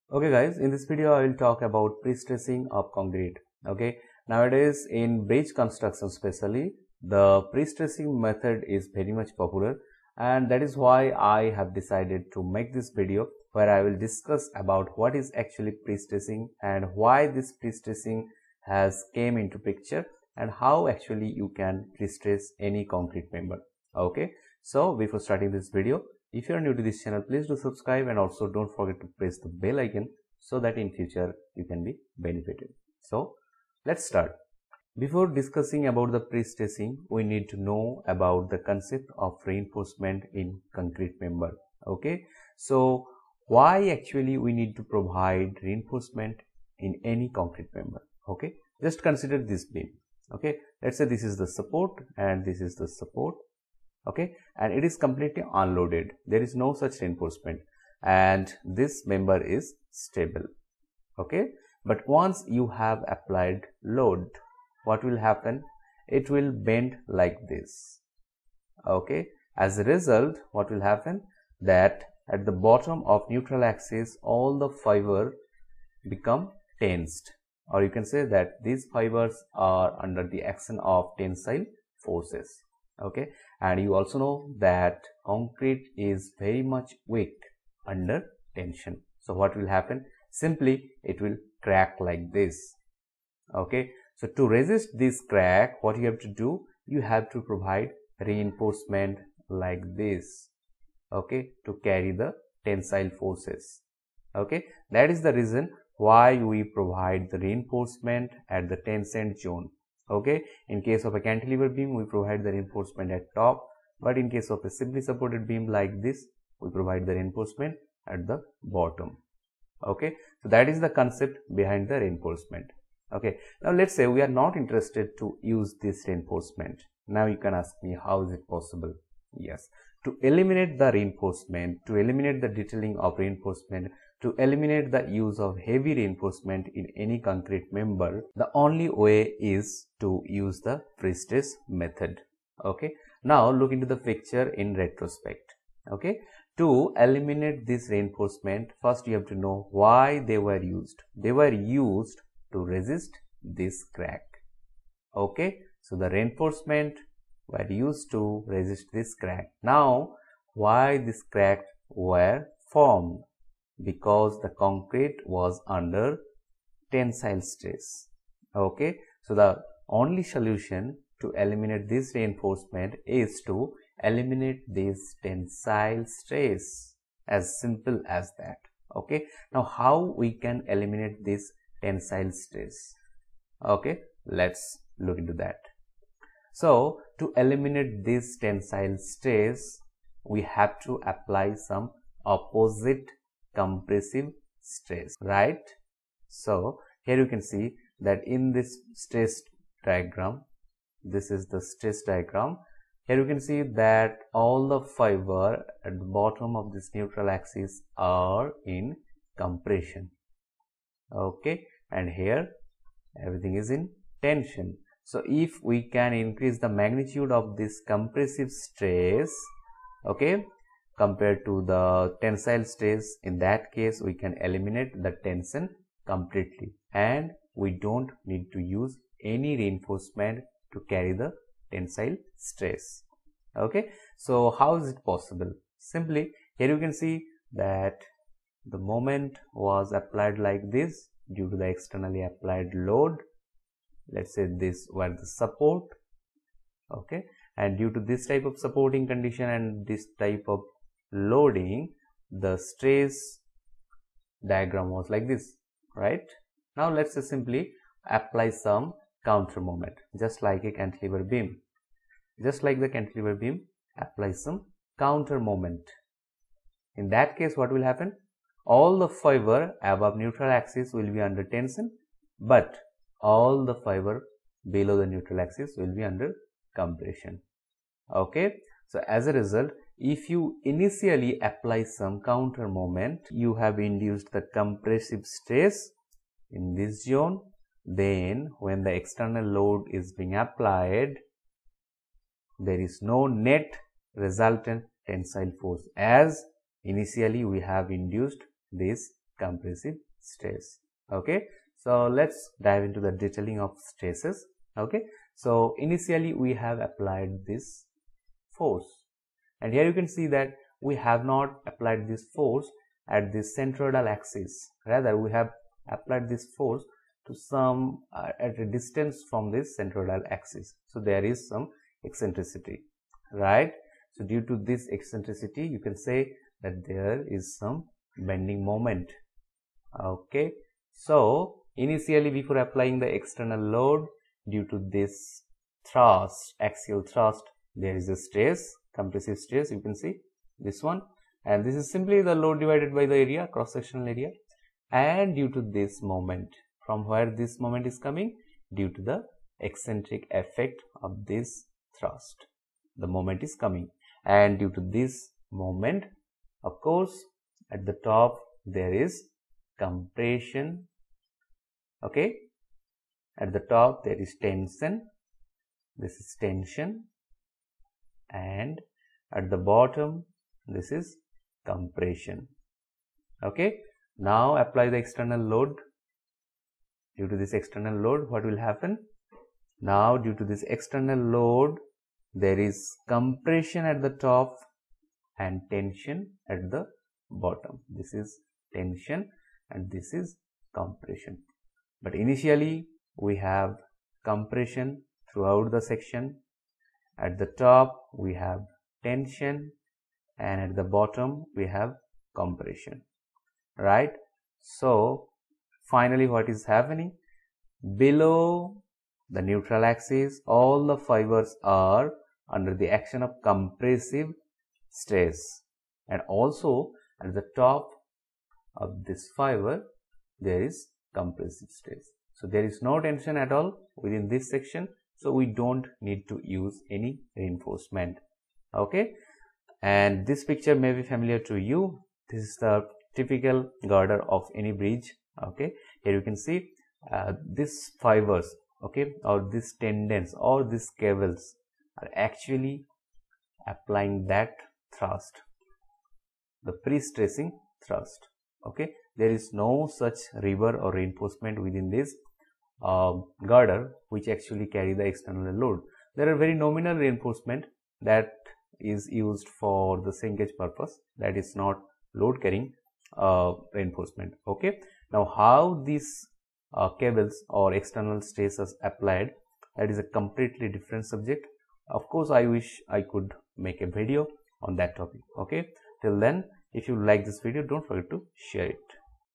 Prestress Concrete : An Overview
Watch this video to lean about the -
Concept of Prestresing,
Why Prestressing is required,
How to Design the prestress concrete and many more.